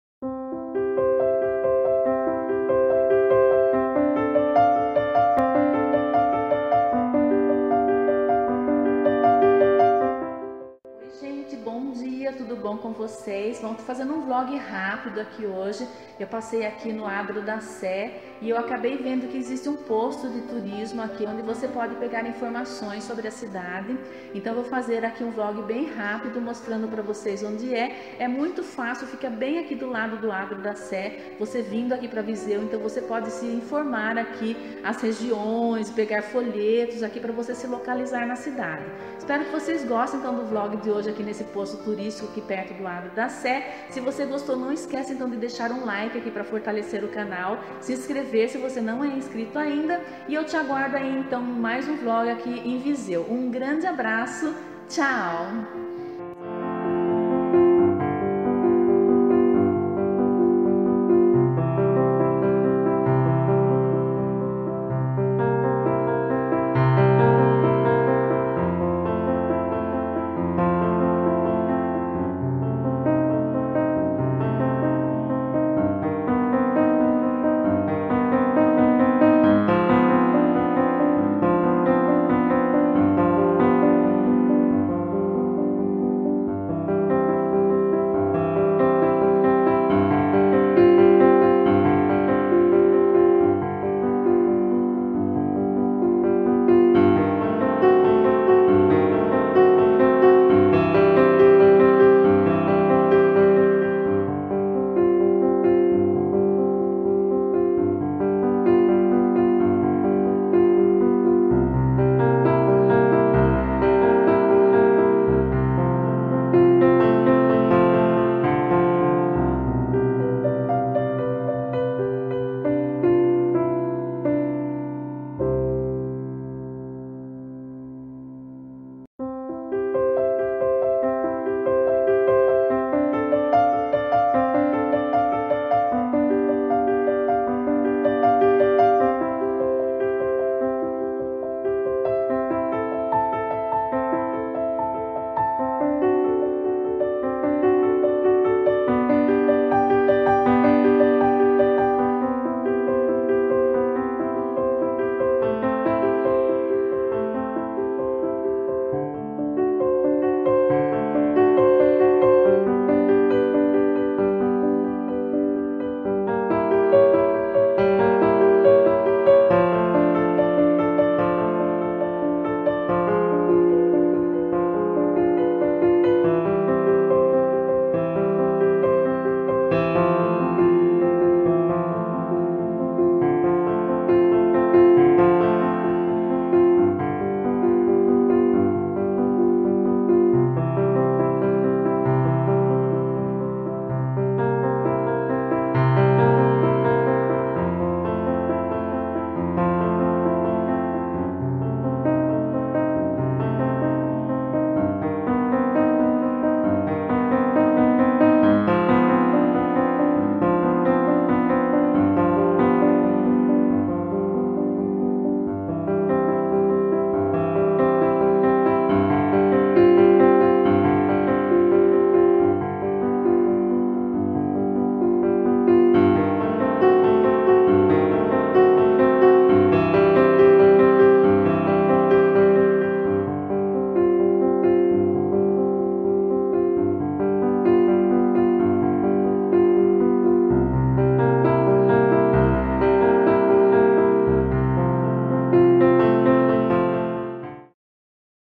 Posto de informações turísticas em Viseu - Minha vida em Portugal
Precisa de informações turísticas? Confira o endereço desse posto fabuloso de turismo de Viseu.